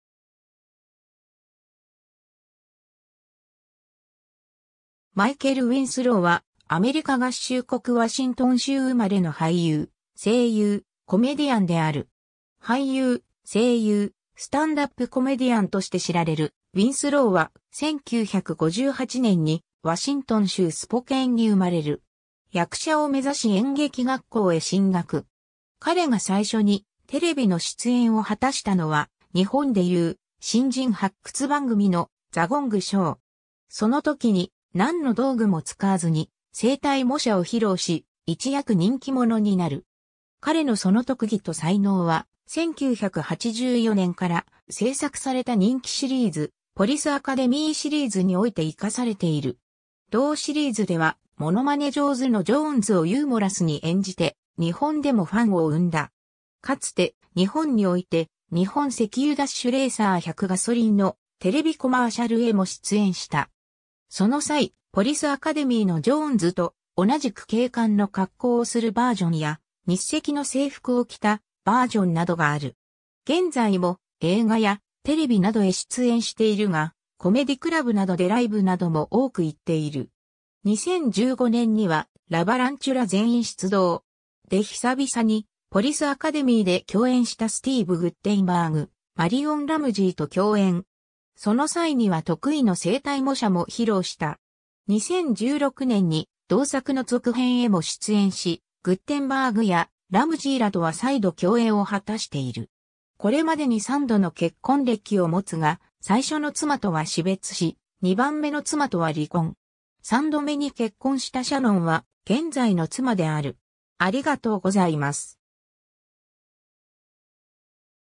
マイケル・ウィンスロー
マイケル・ウィンスロー（Michael Winslow、1958年9月6日 - ）は、アメリカ合衆国ワシントン州生まれの俳優・声優・コメディアンである。
 俳優、声優、スタンダップコメディアンとして知られるウィンスローは、1958年にワシントン州スポケーンに生まれる。
役者を目指し演劇学校へ進学。
彼が最初にテレビの出演を果たしたのは、日本で言う新人発掘番組の“ザ・ゴング・ショー”。
その時に、何の道具も使わずに声帯模写を披露し、一躍人気者になる。
彼のその特技と才能は、1984年から製作された人気シリーズ『ポリスアカデミーシリーズ』において活かされている。
同シリーズでは物まね上手のジョーンズをユーモラスに演じて、日本でもファンを生んだ。
かつて日本において“日本石油ダッシュレーサー100ガソリン”のテレビコマーシャルへも出演した。
その際、ポリスアカデミーのジョーンズと同じく警官の格好をするバージョンや、日石の制服を着たバージョンなどがある。
現在も映画やテレビなどへ出演しているが、コメディクラブなどでライブなども多く行っている。
 2015年には、『ラバランチュラ 全員出動!』で久々に『ポリス・アカデミー』で共演したスティーヴ・グッテンバーグ、マリオン・ラムジーと共演。
その際には得意の声帯模写も披露した。
2016年に同作の続編へも出演し、グッテンバーグやラムジーらとは再度共演を果たしている。
 これまでに3度の結婚歴を持つが、最初の妻とは死別し、2番目の妻とは離婚。
3度目に結婚したシャノンは現在の妻である。